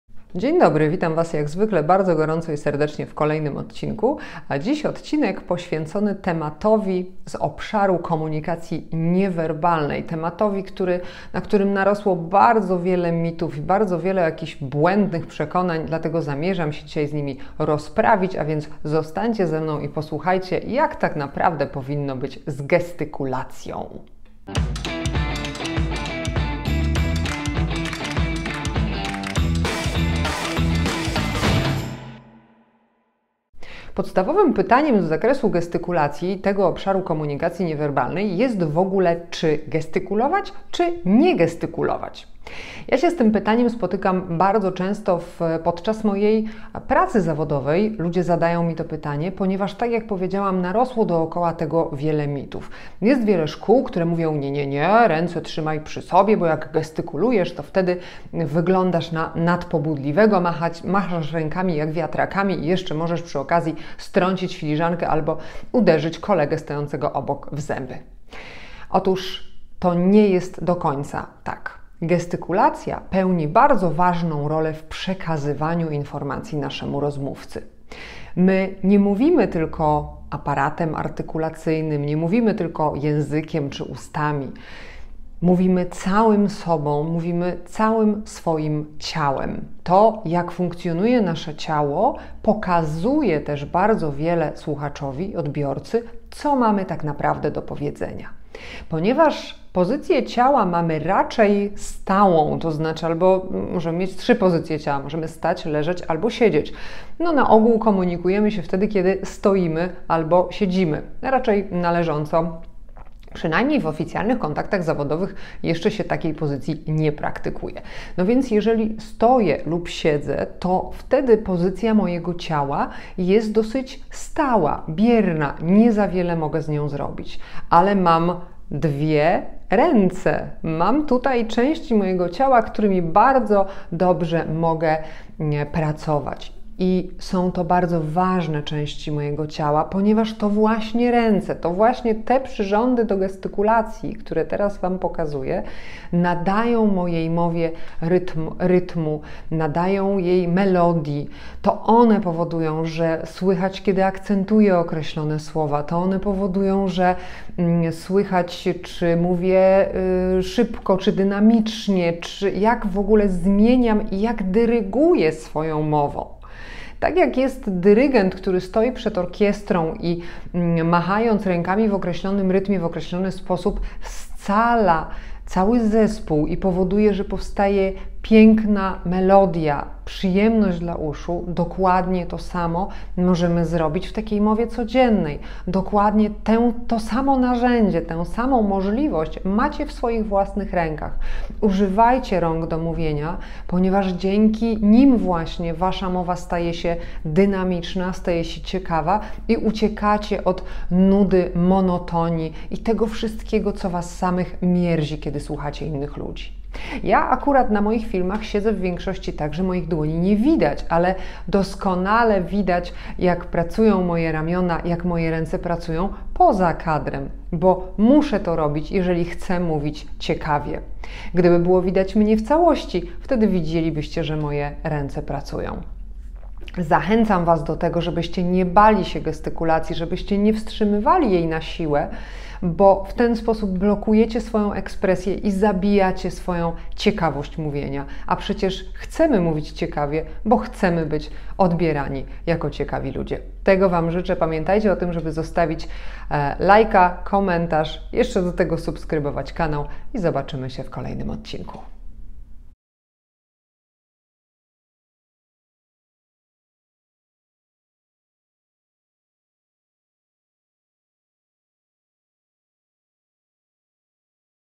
Gestykulacja #019
Gestykulacja nie tylko przydaje się w komunikacji ale jest w niej wręcz niezbędna! Ręce to nasz "dyrygent", który decyduje o tempie i melodii wypowiedzi. Dzięki pracy rąk nasza mowa jest dynamiczna i ciekawa. Warto o tym pamiętać oraz... używać rąk do mówienia :-)